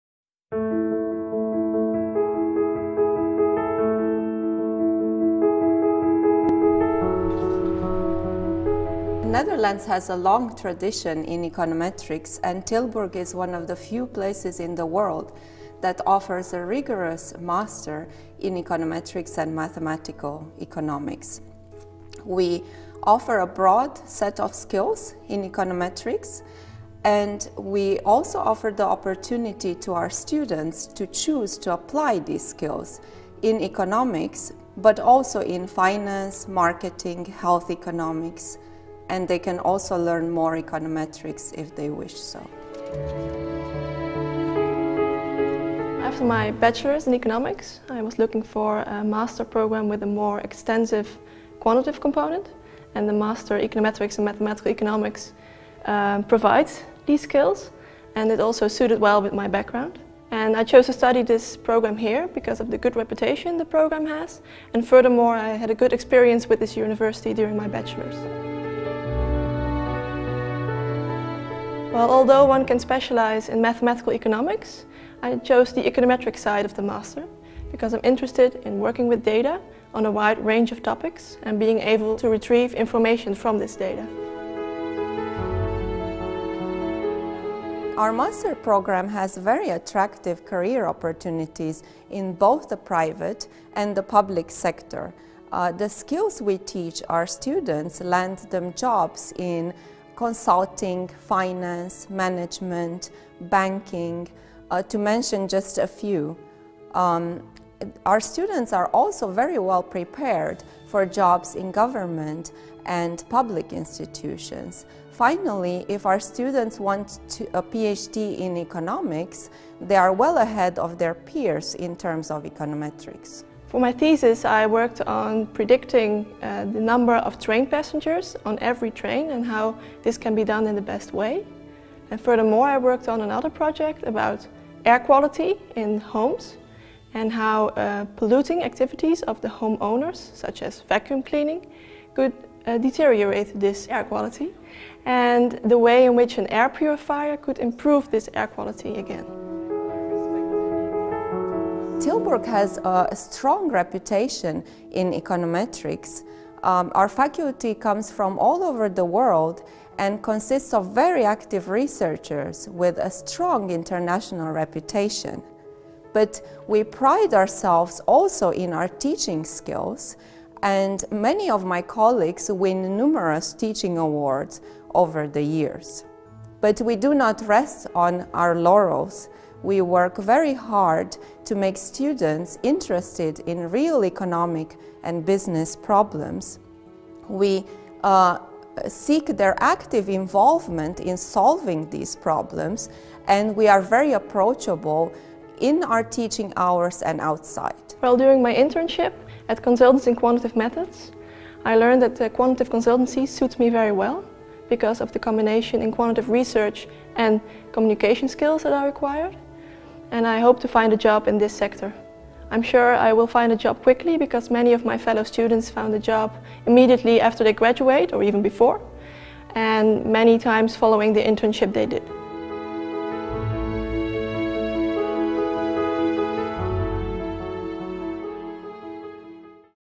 Master Econometrics and Mathematical Economics - Tilburg University
Taught by some of Europe's pre-eminent academics in the field of econometrics, the Master in Econometrics and Mathematical Economics at Tilburg University focuses on the mathematical and quantitative analysis of economic problems. Acquire the tools to apply mathematical and statistical methods to produce solutions to real-world economic and business challenges in everything from the labor market, capital investment and industrial organizations, to the strategic behavior of firms and individual decision-making!

Learn more about this Master's program by watching the video.